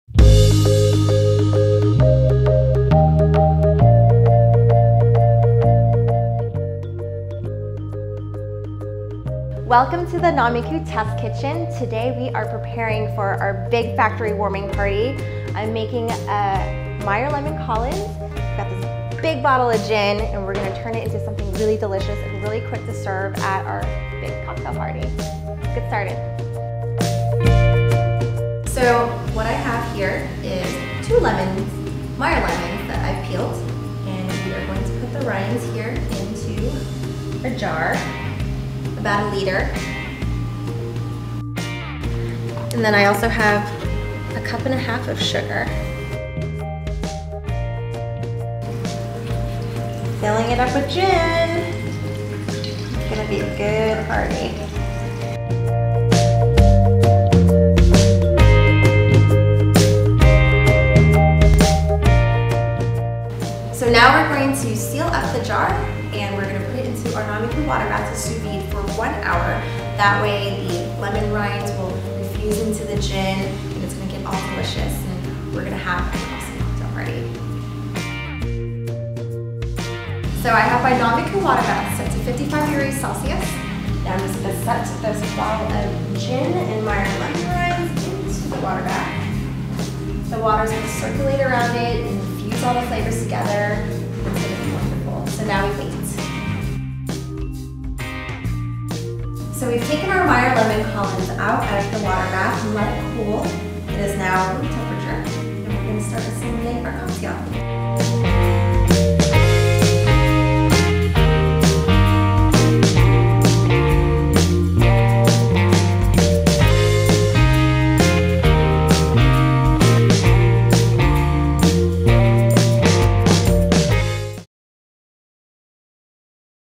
Sous Vide Meyer Lemon Collins
This drink will take the edge off, while refreshing you all the same. Get your weekend started early.

Makes 1.75 L
Cook time: 1 Hour Sous Vide

Ingredients:
1.75L Gin
2 Cups Meyer Lemon Rinds (About 6 Meyer Lemons)
2 Meyer Lemons - Juiced
1.5 Cups White Granulated Sugar
Soda Water to Serve
Ice
Thyme Sprigs

Steps:
1. Set up Nomiku water bath and turn temperature to 55°C (131°F).

2. In a sealable large glass container, fill with gin, Meyer Lemon rinds, and sugar. Seal and sous vide for 1 hour.

3. After 1 hour, remove from Nomiku water bath and let cool.

4. To serve, fill a tall glass half full of ice, add an ounce of the infused gin, 1/2 oz of Meyer Lemon juice, and fill to the top with soda water. Garnish with a Meyer lemon rind. Take a sprig of thyme and smack it in between your hands to release aromas and place on top of drink to finish.